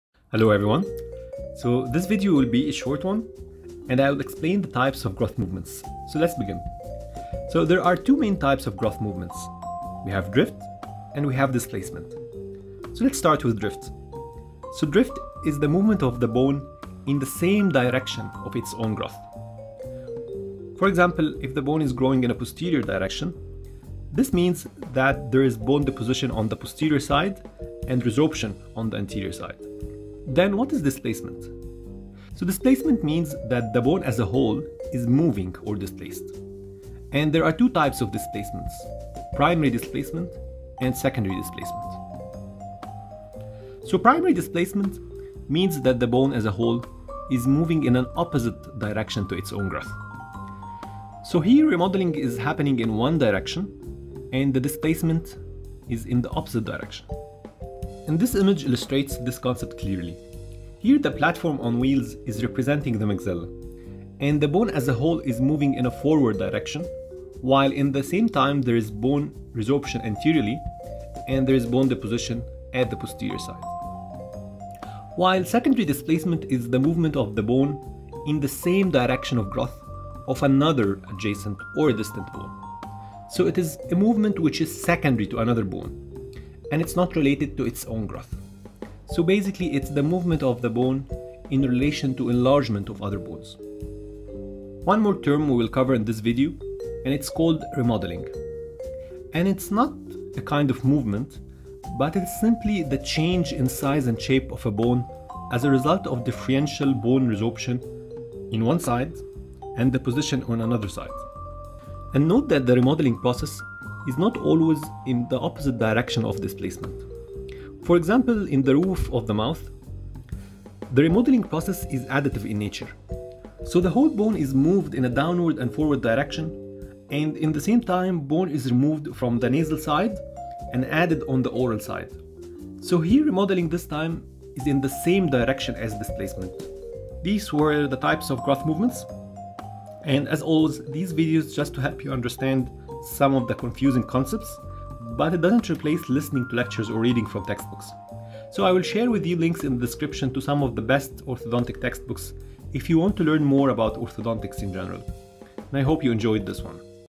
Types of growth movements in Orthodontics
Drift, 1ry displacement and 2ry displacement

Books to consider:
- Contemporary Orthodontics -  William Profitt

- An Introduction to Orthodontics -   Simon J. Littlewood , Laura Mitchell